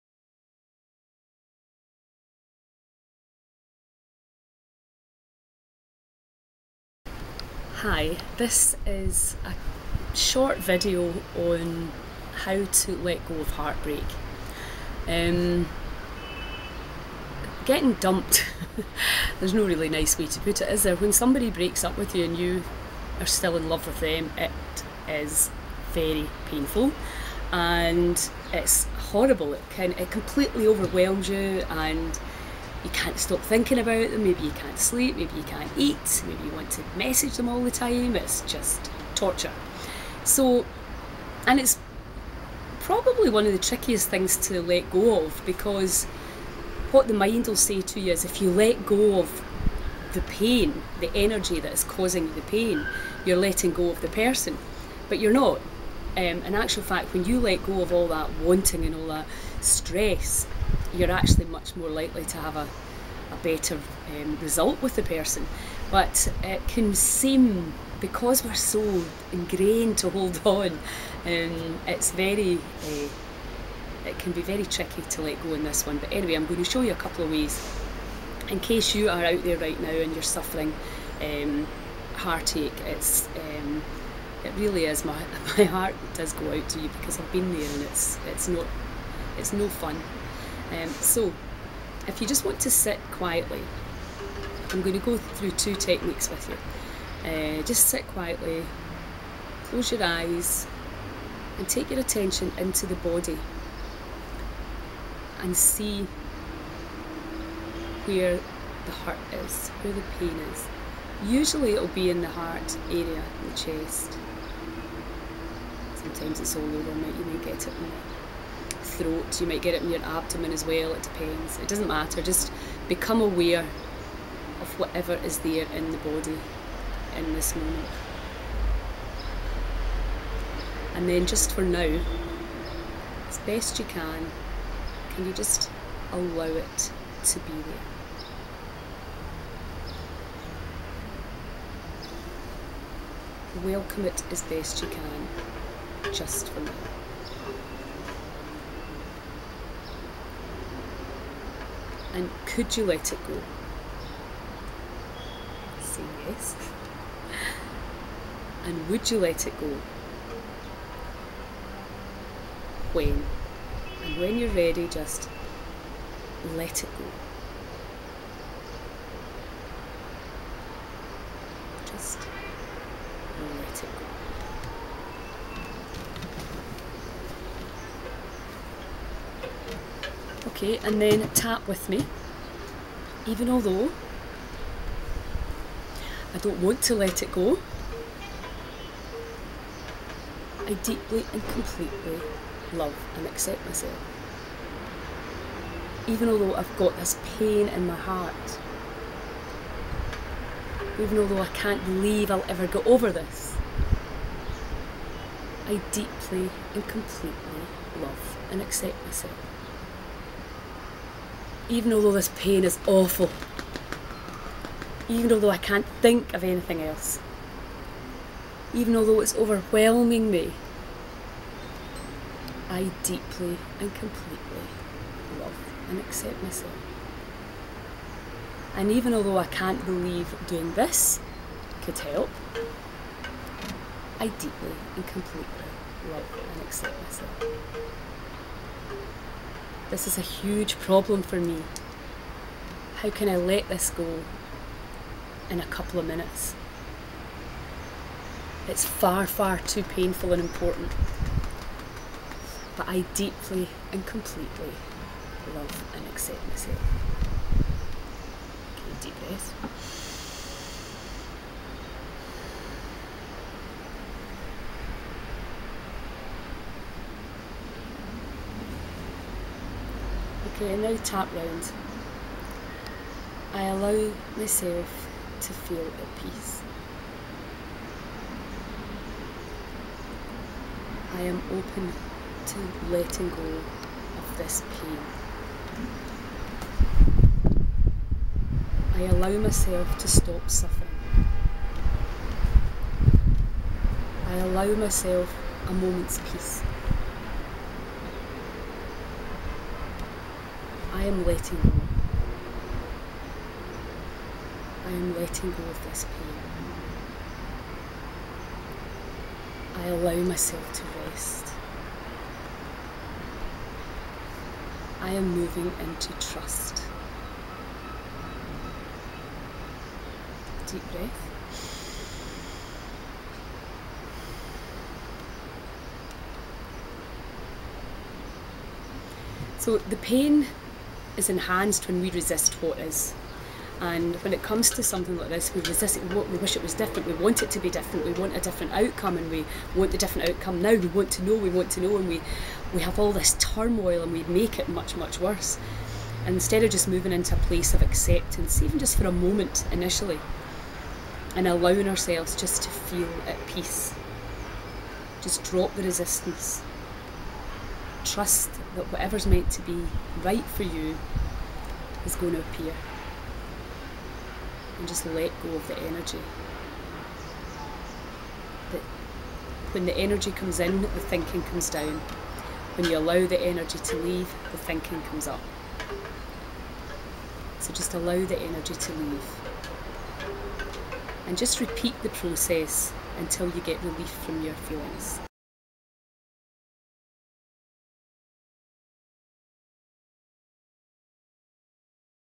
Letting go of a broken heart
Getting dumped can be one of the most horrific experiences you will ever go through. The emotional pain can be debillitating and can erase a couple of fun years out your life.
When you are fed up with this pain, you can let it go.